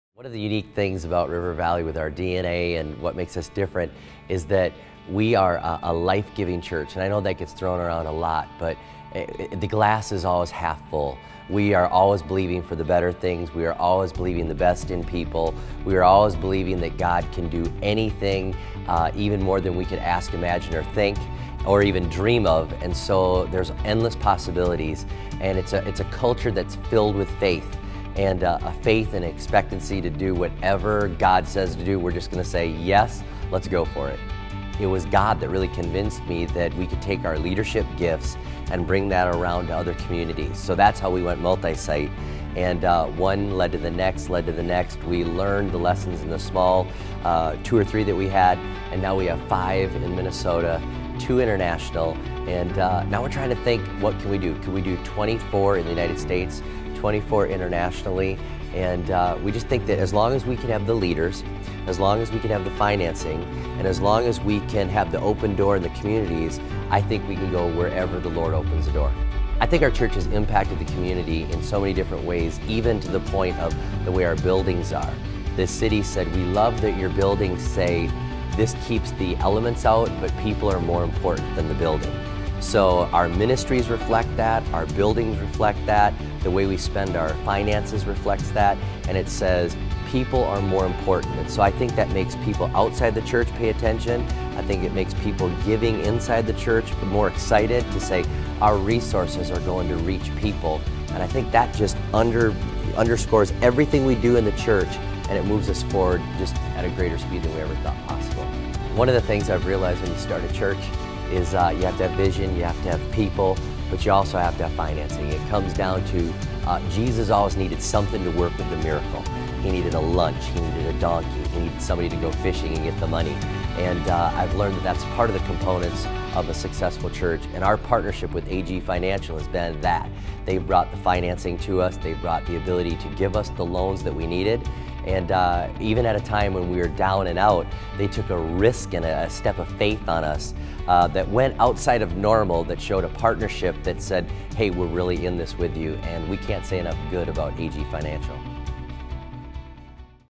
River Valley Church | Apple Valley, MN
Rob Ketterling, lead pastor at River Valley Church, explains the impact that their relationship with AG Financial Solutions has had on their ministry.